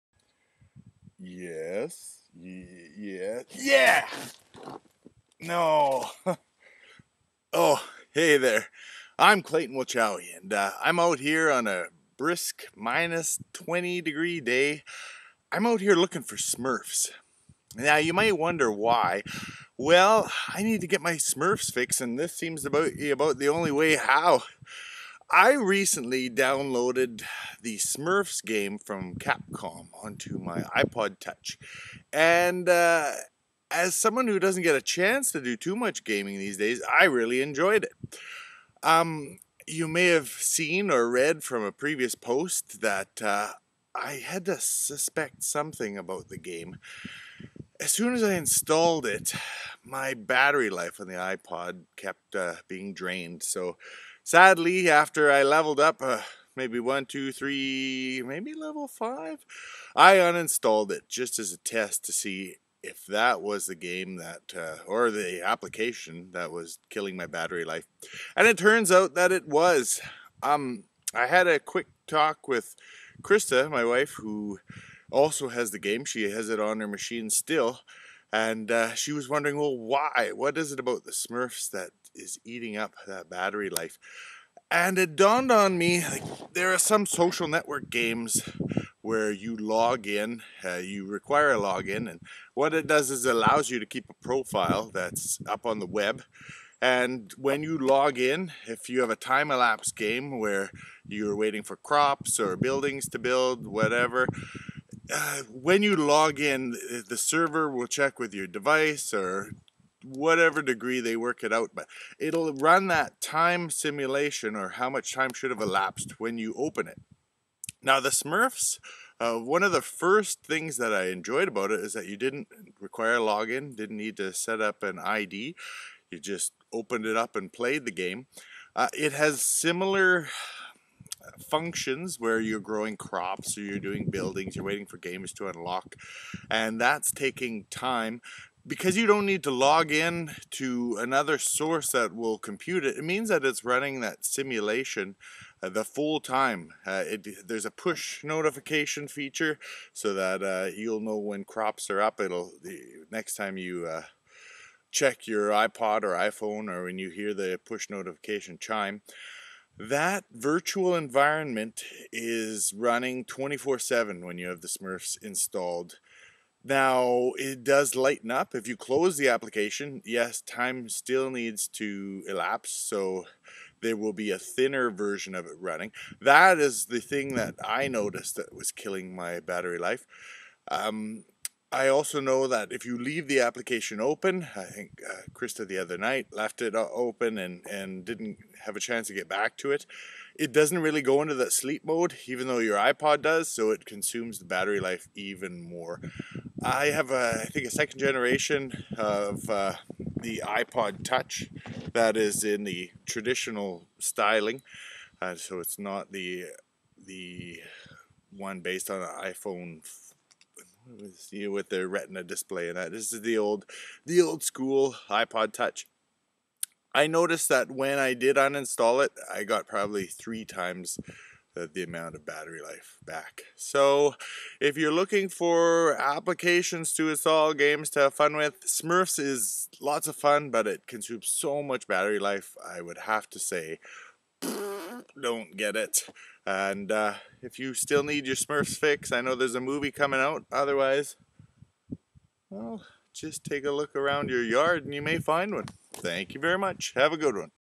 Smurfs App for iPhone/touch a battery vampire
Clayton Wilchowy explains why he had to uninstall the Capcom Smurfs game off his iPod touch due to the fact that it drains battery life. He explains a bit about the local simulation (a little benefit, unfortunately a lot of battery bad!)